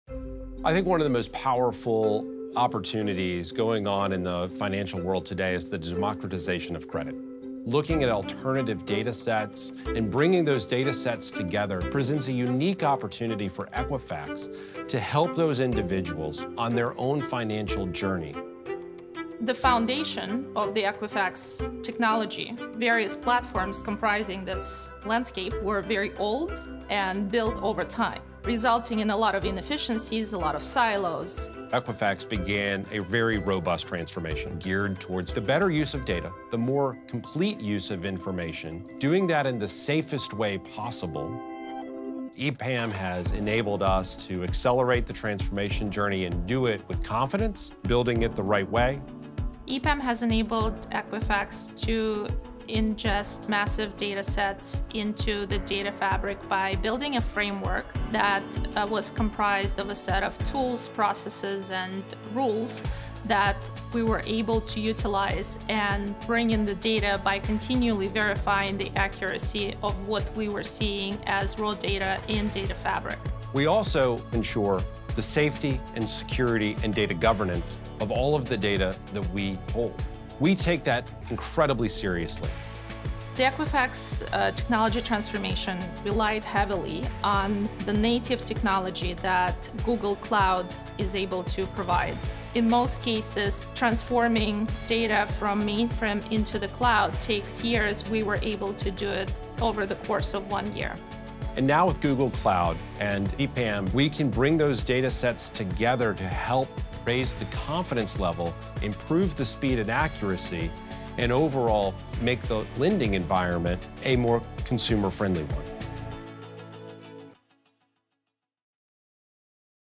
How Equifax democratizes credit access with help from Google Cloud and EPAM Systems
Learn how Equifax's move to Google Cloud with help from EPAM Systems enabling the company to develop innovative data-driven products and services that help its customers live their financial best.